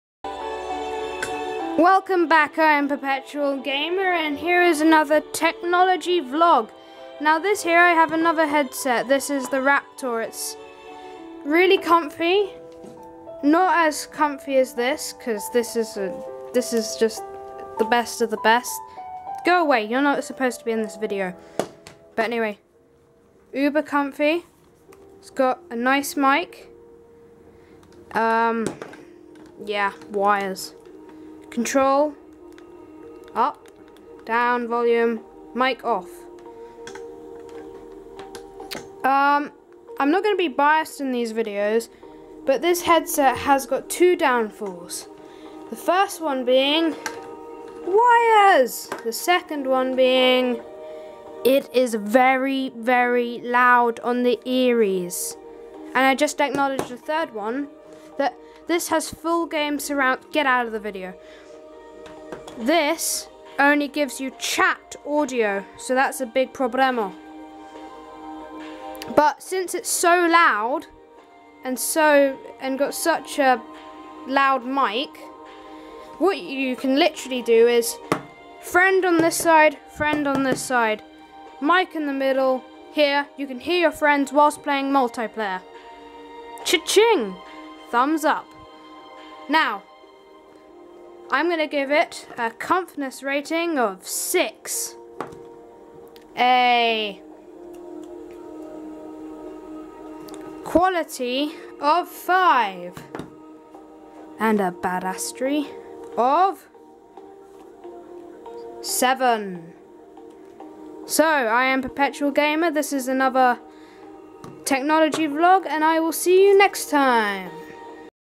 Tech Bites: Raptor 2450
Important below:

Today Perpetual reviews the Raptor 2450!

All my videos are protected under copyright law!